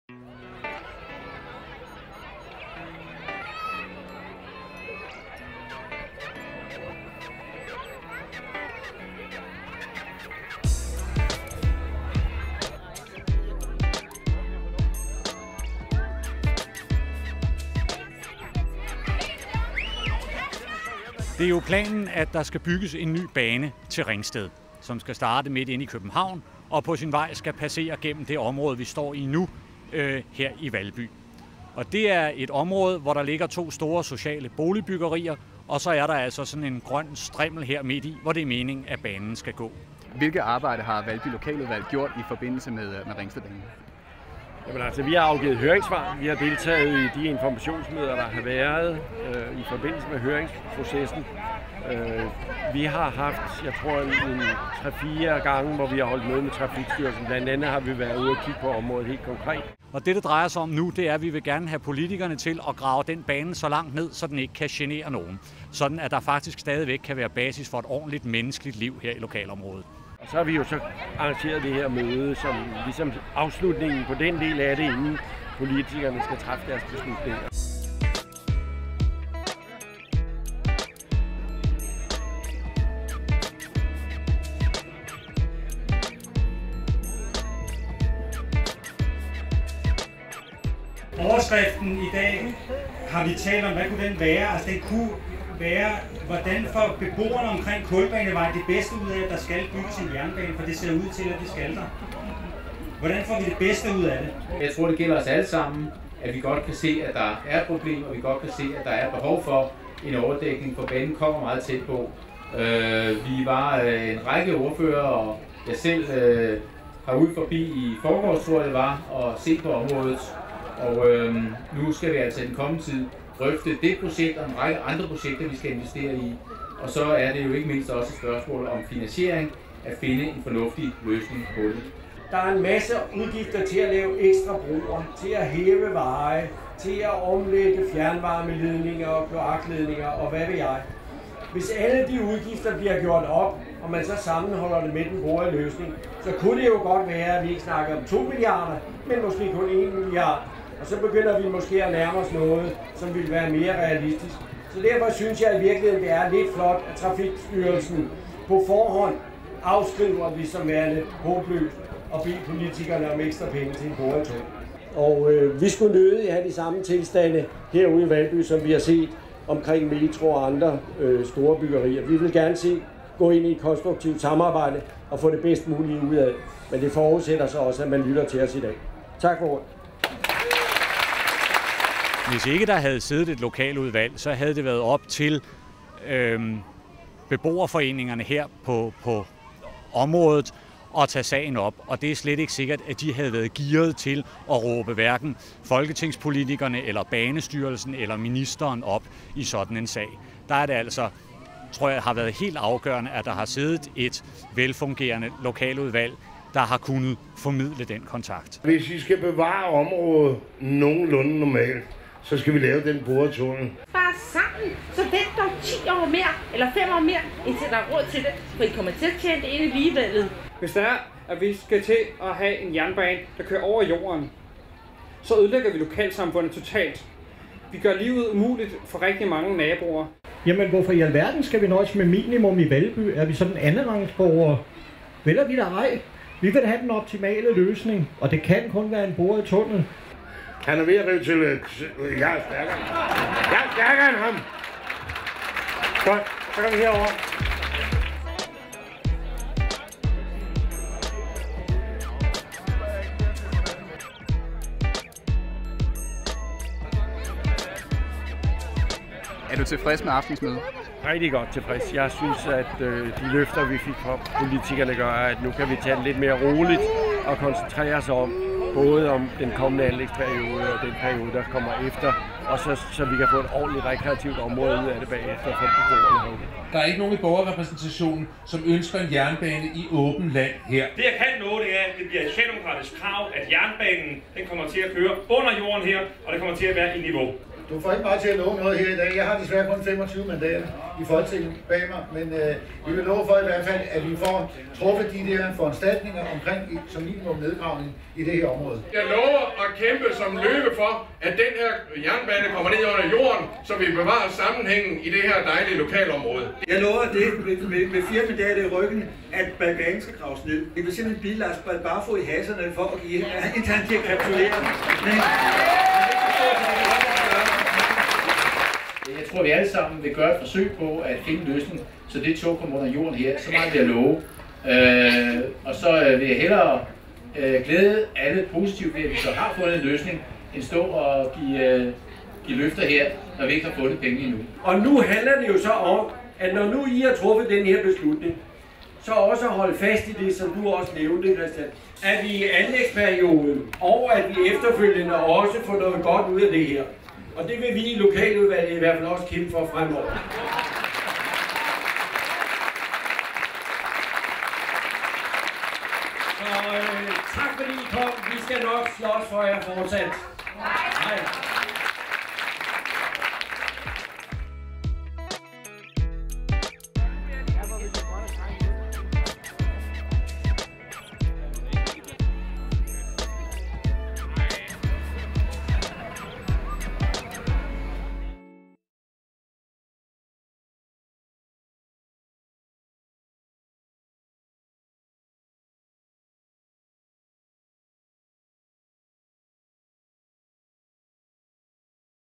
Valby Lokaludvalg - Borgermøde om Ringstedbanen
Valby risikerer at blive særlig hårdt ramt af en kommende jernbanefor-bindelse mellem København og Ringsted over Køge. Banen skal sand-synligvis føres tæt forbi flere boligområder, og løbe på arealer, der i dag anvendes til rekreative formål.